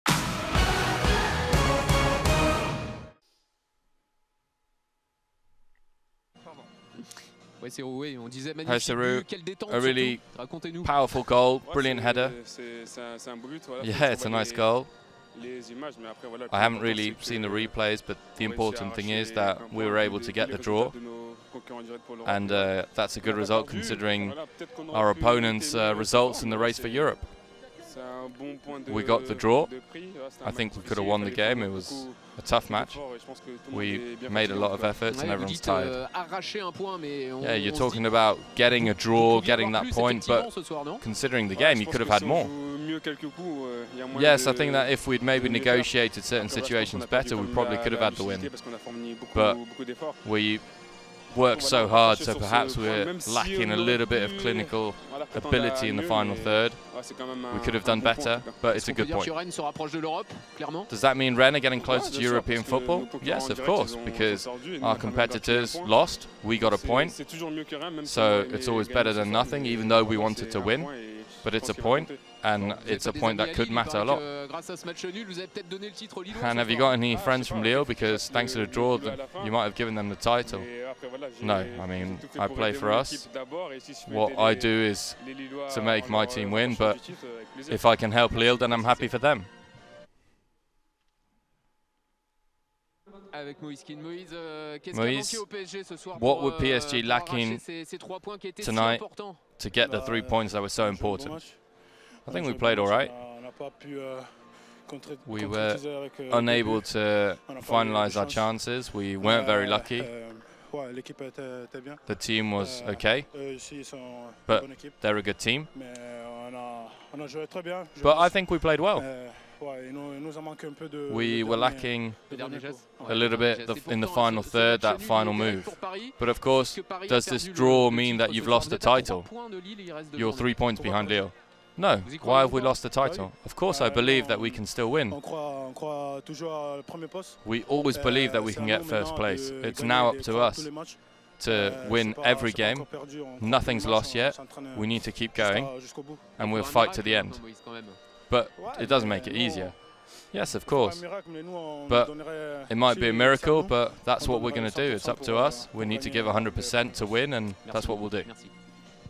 Reaction : STADE RENNAIS FC - PARIS SAINT-GERMAIN (1-1) / Week 36 - Ligue 1 Uber Eats / 2020-2021
Watch reaction after STADE RENNAIS FC - PARIS SAINT-GERMAIN (1-1) in video.

Ligue 1 Uber Eats - Season 2020/2021 - Week 36 ROAZHON PARK - Sunday 9 May 2021

Goals :  NEYMAR JR (45' +6 pen - PARIS SAINT-GERMAIN) / Serhou GUIRASSY (70' - STADE RENNAIS FC) Coaches : Bruno GENESIO(STADE RENNAIS FC) - Mauricio Roberto POCHETTINO(PARIS SAINT-GERMAIN)